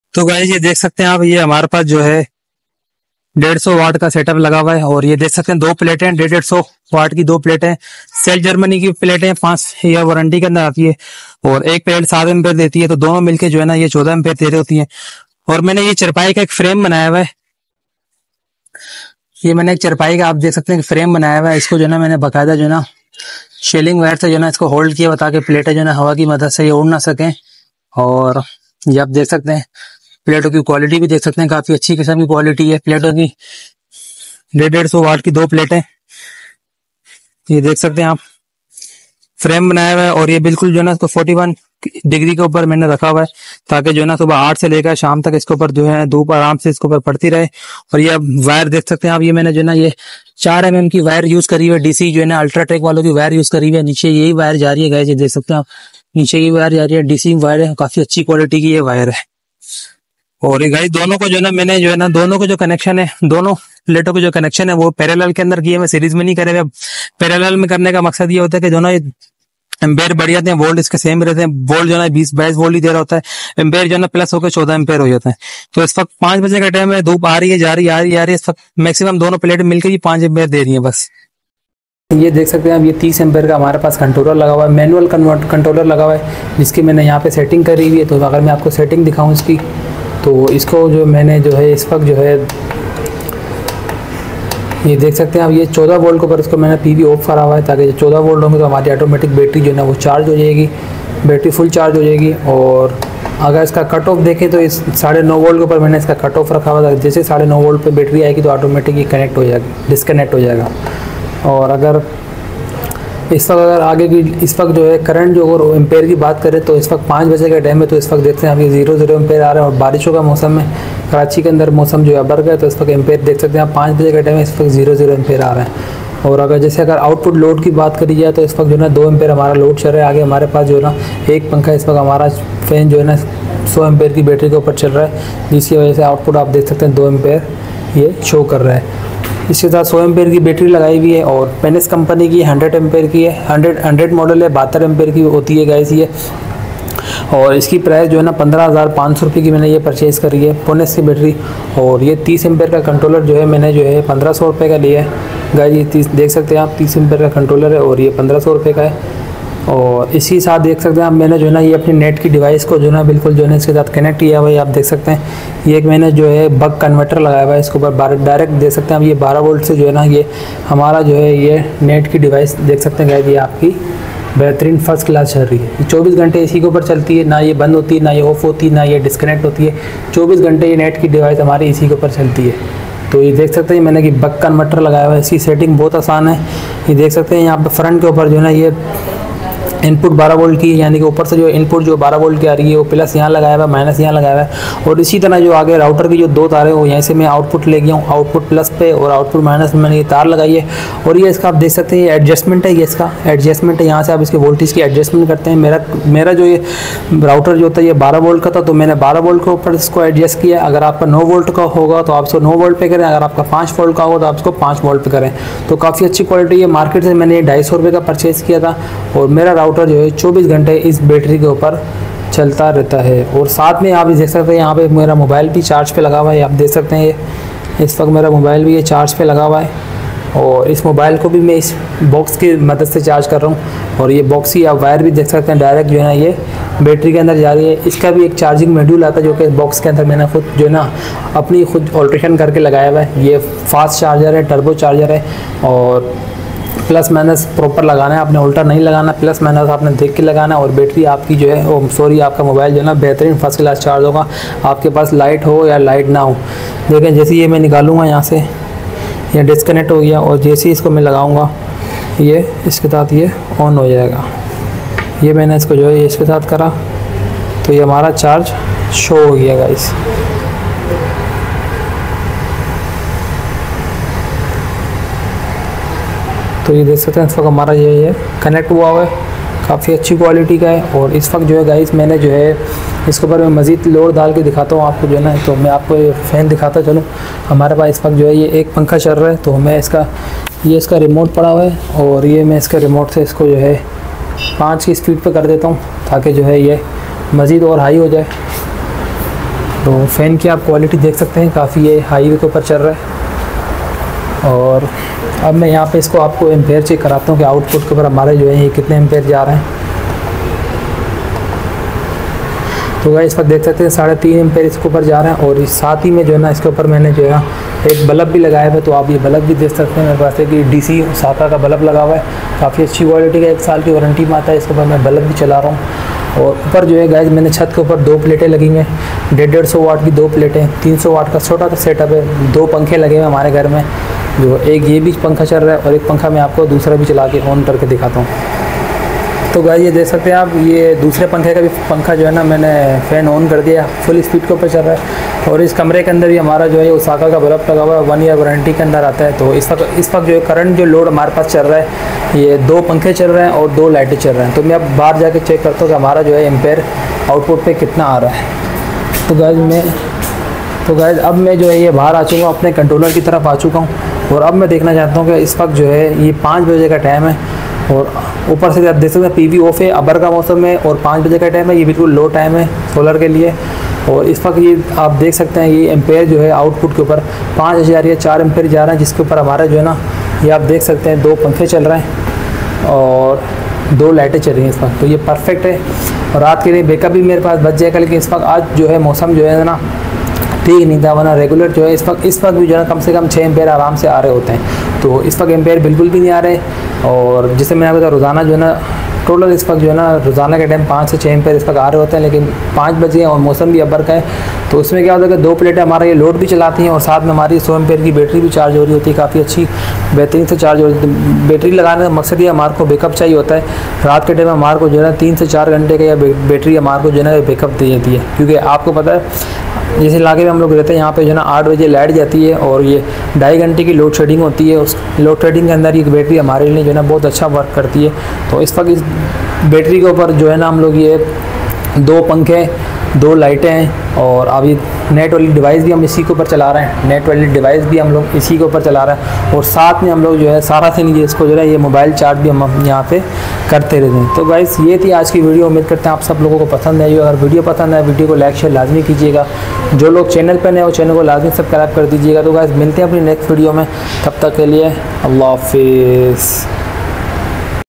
300 WATS SOLAR PANELS PE | AC DC FANS 2 DC LED BULBS|WIFI OR MOBILE CHARGING SARA DIN FREE ME CHALAI
300 WATS SOLAR PANELS PE | AC DC FANS 2 DC LED BULBS|WIFI OR MOBILE CHARGING SARA DIN FREE ME CHALAI